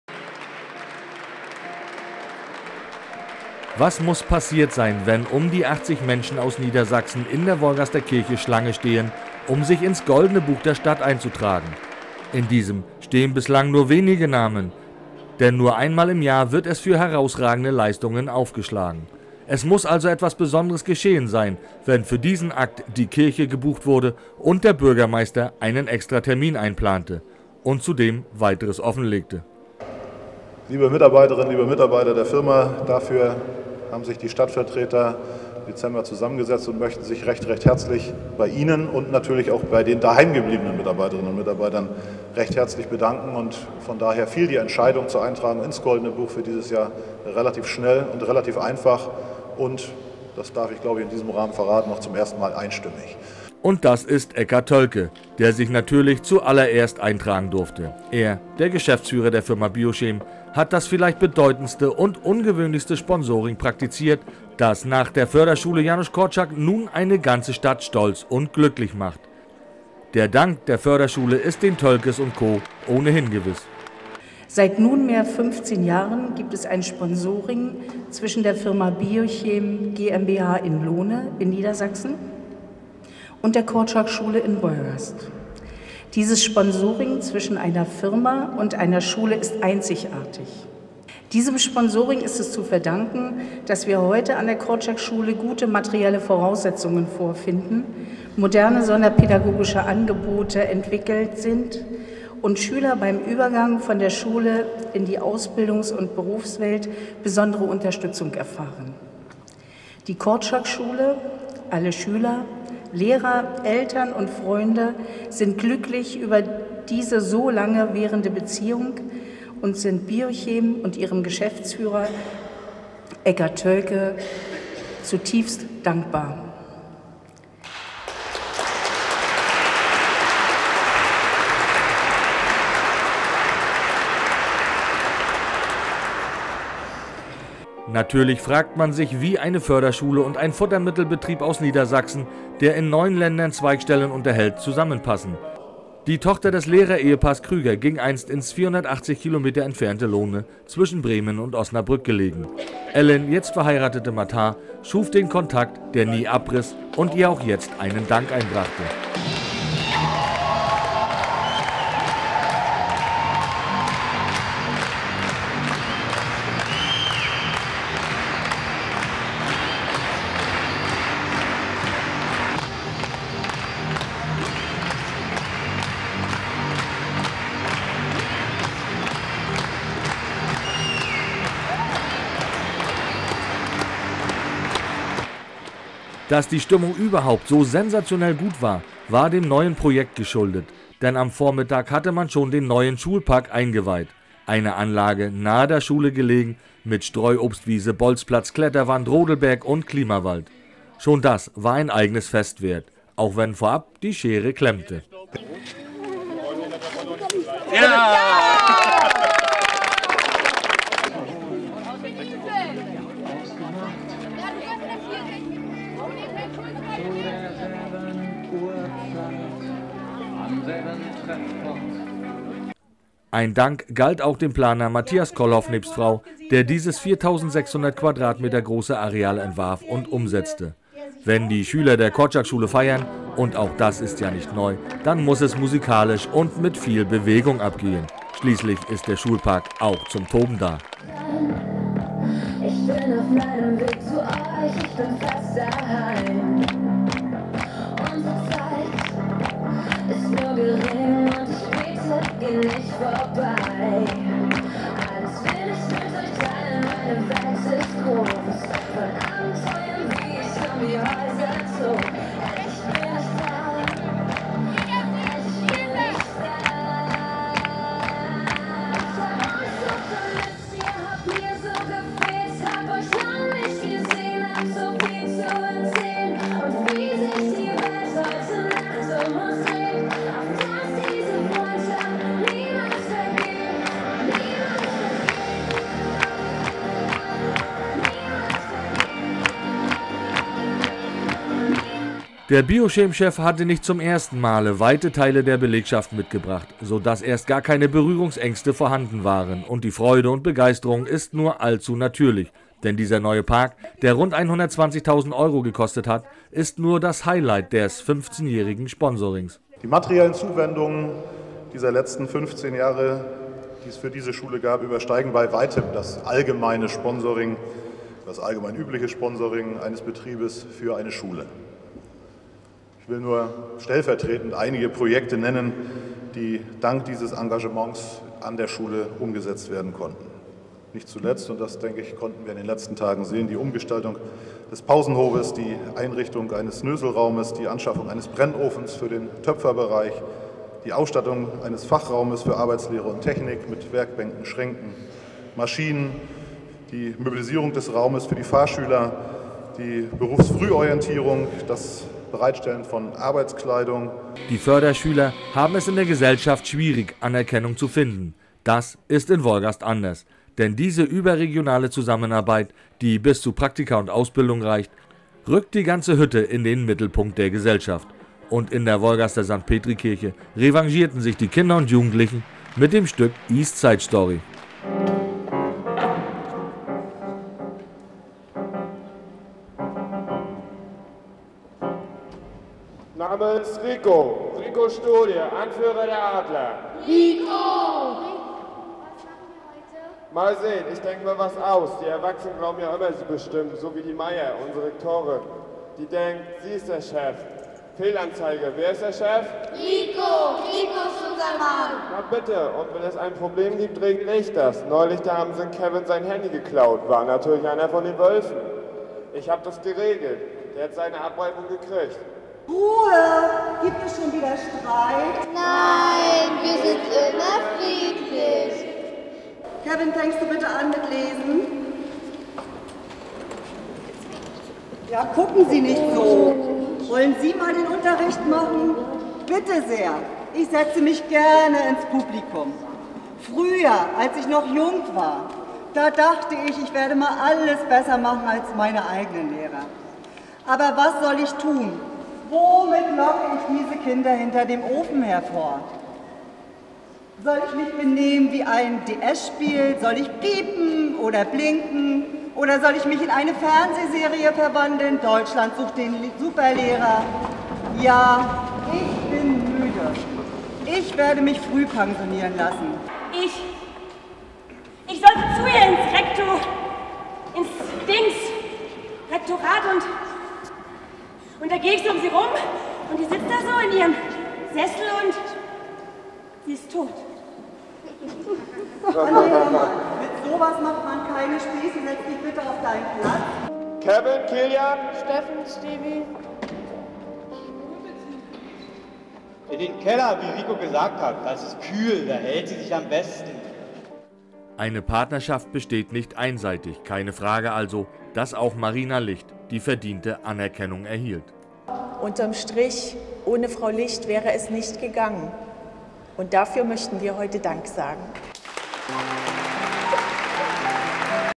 CSR Project: Biochem supports Janusz Korczak School
“We may not be able to prepare the future for our children, but we can at least prepare our children for the future.” – Franklin Roosevelt –

Changing children’s lives; preparing them for the future seems more than comprehensive.

Being socially responsible, it’s a belief that Biochem not only shares but truly acts on.

By supporting Janusz Korczak School in Wolgast with learning material and more educational friendly environment  for over 20 years, the company demonstrates its commitment to social development.